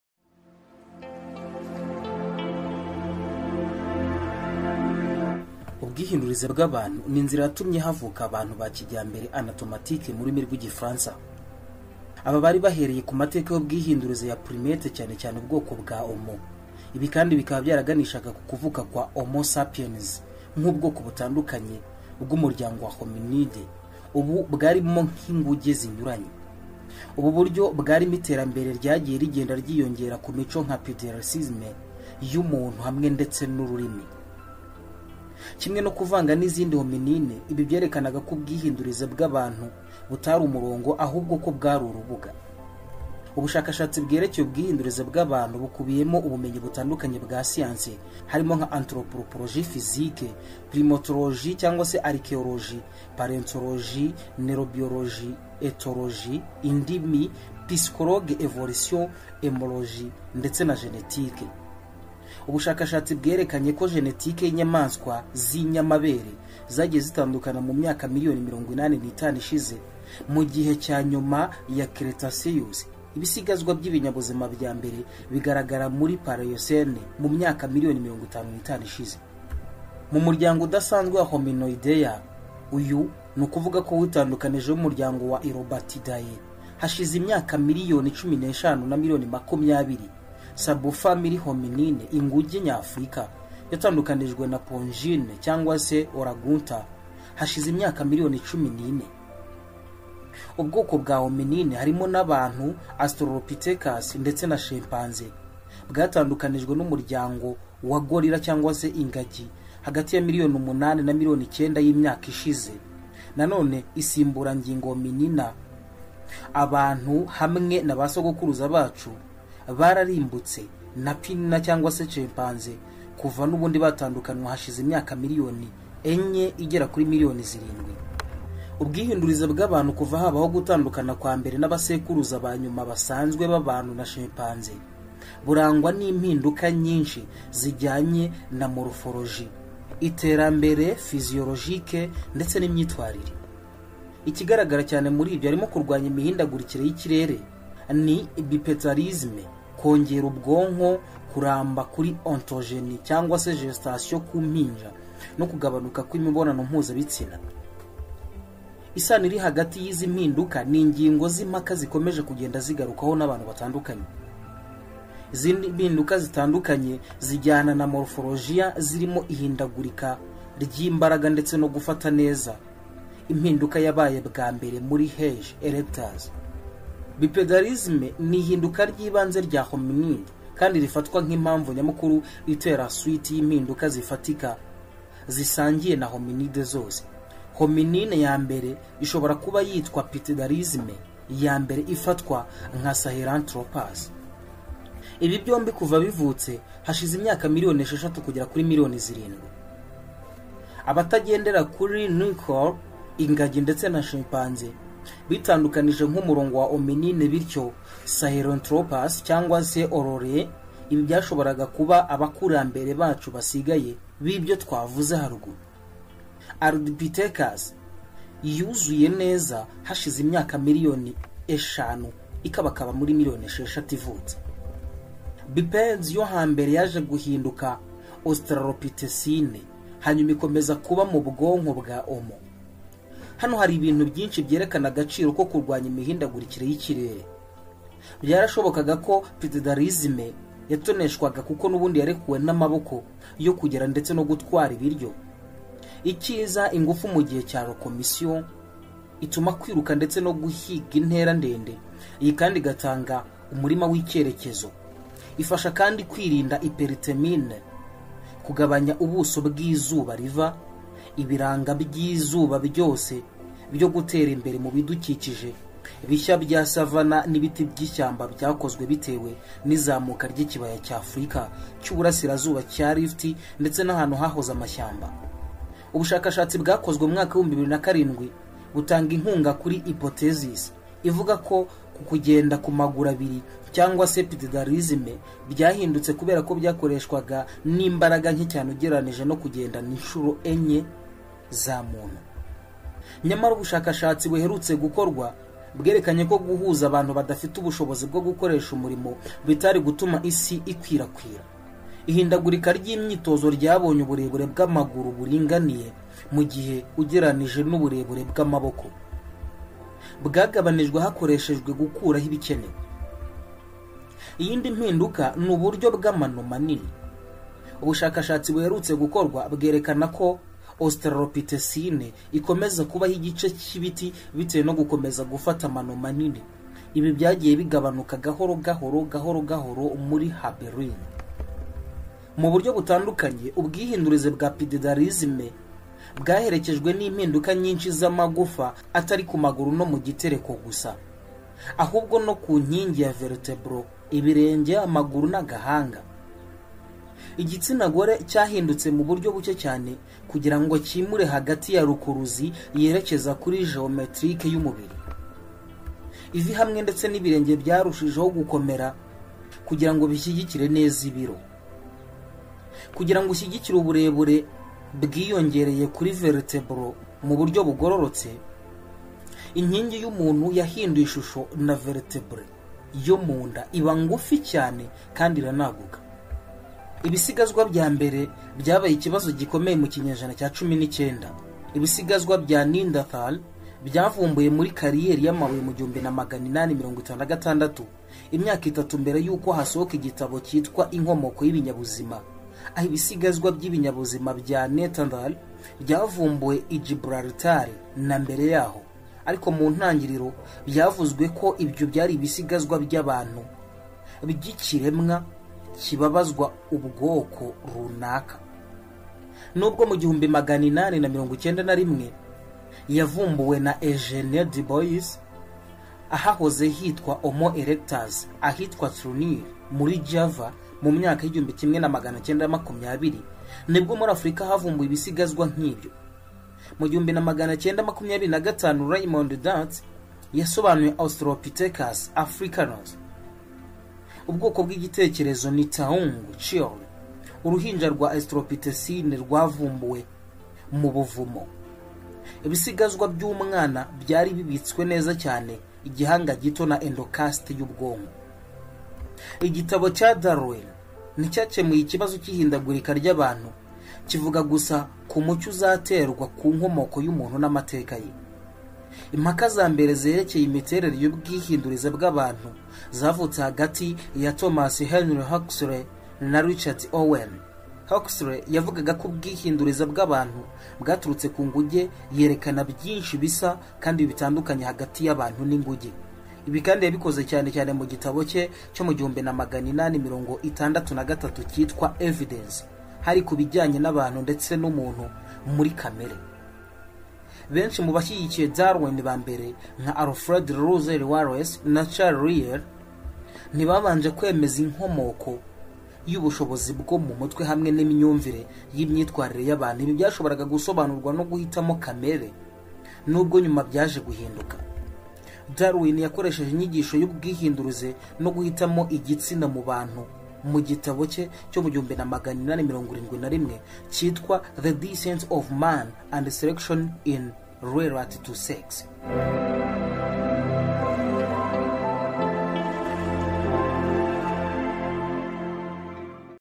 SOBANUKIRWA UBWIHINDURIZE BW'ABANTU(HUMAN EVOLUTION)😎😎
munkuru yacu turagaruka ku mateka y'ubwihindurize bwa muntu